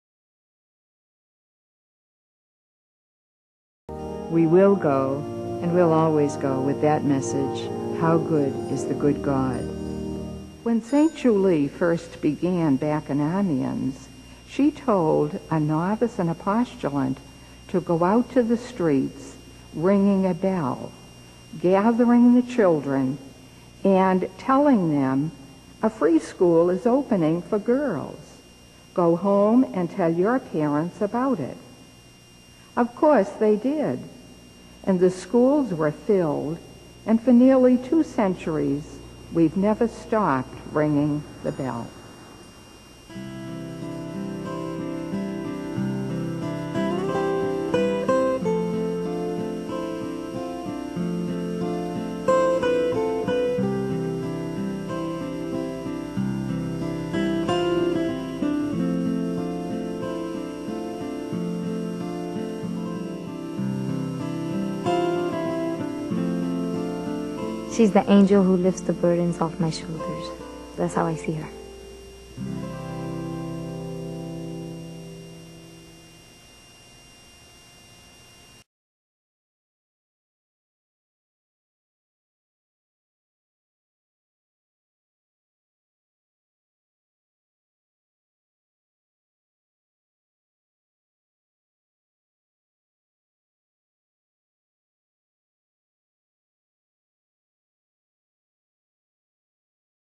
St Julie and Education -- the first schools and beyond.wmv
Sr. Julienne Sullivan tells the story of the first school opened by the Sisters of Notre Dame de Namur in Amiens, France.